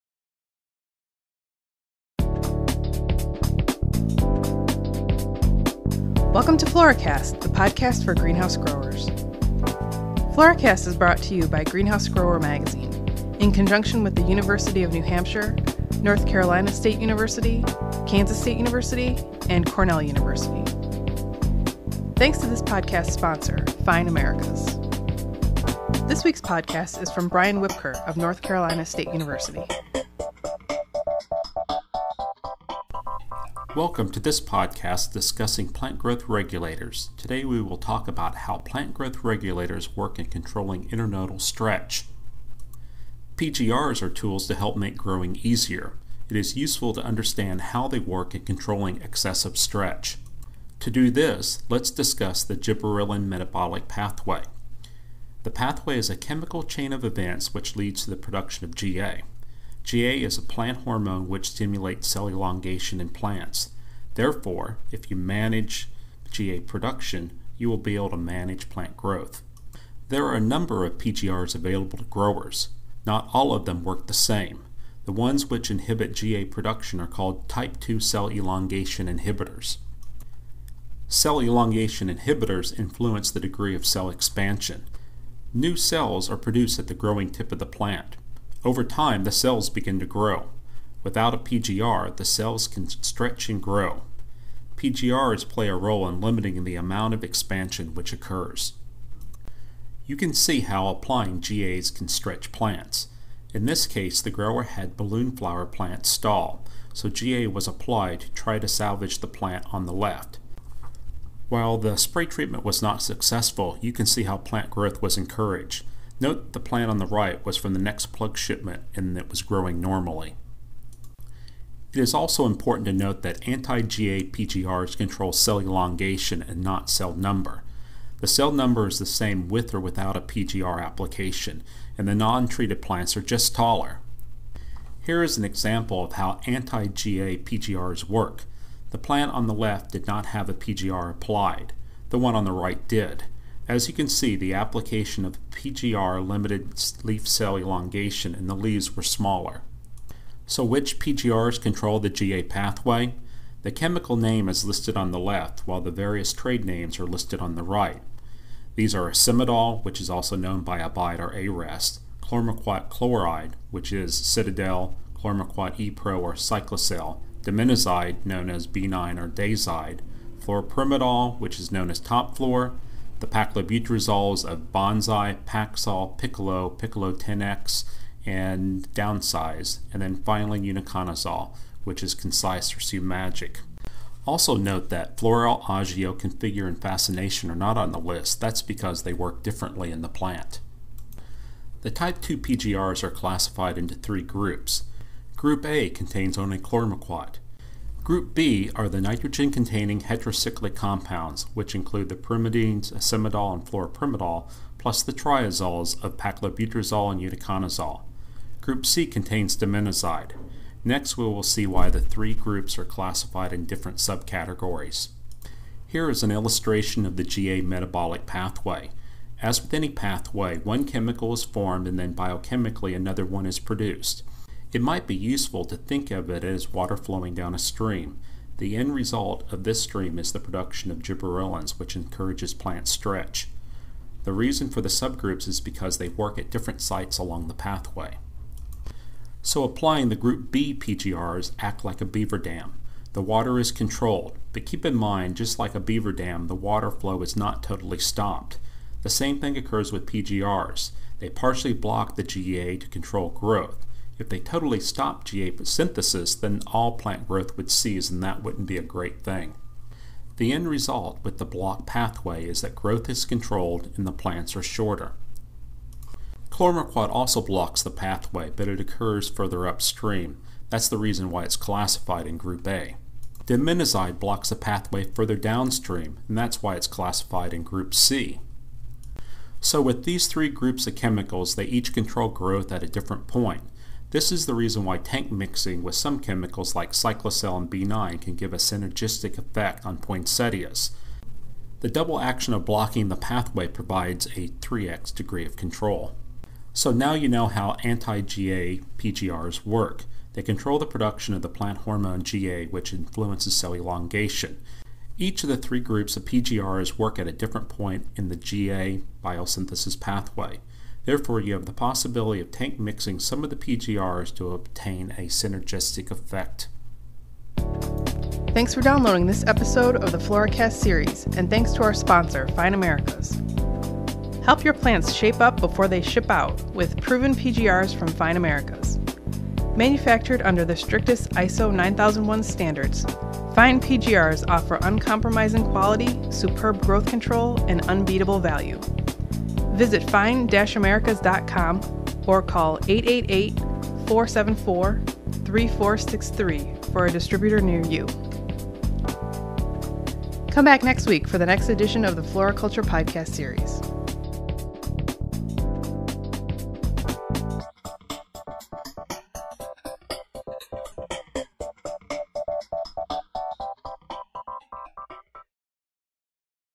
How Plant Growth Regulators Work
Hosted by Dr. Brian Whipker